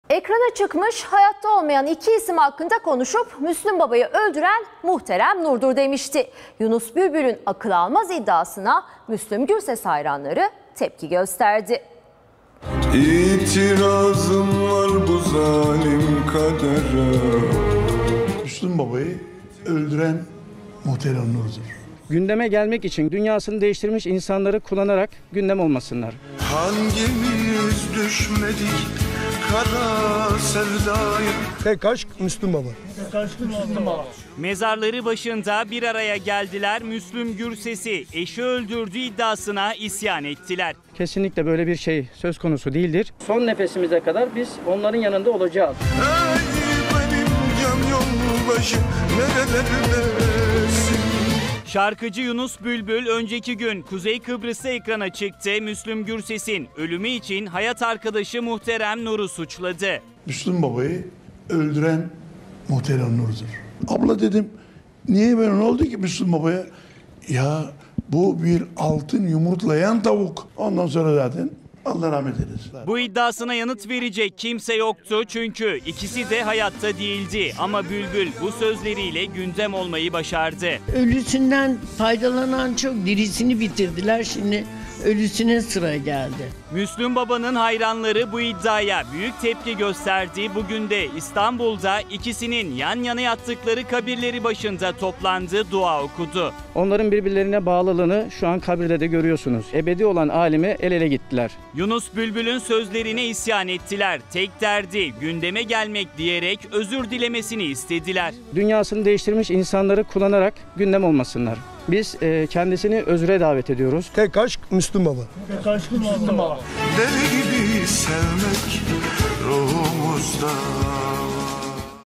Müslüm Baba hayranları isyanda
EKRANA ÇIKMIŞ, HAYATTA OLMAYAN İKİ İSİM HAKKINDA KONUŞUP "MÜSLÜM BABA'YI ÖLDÜREN MUHTEREM NUR"DUR  DEMİŞTİ. YUNUS BÜLBÜL'ÜN AKIL ALMAZ İDDİASINA MÜSLÜM GÜRSES HAYRANLARI TEPKİ GÖSTERDİ.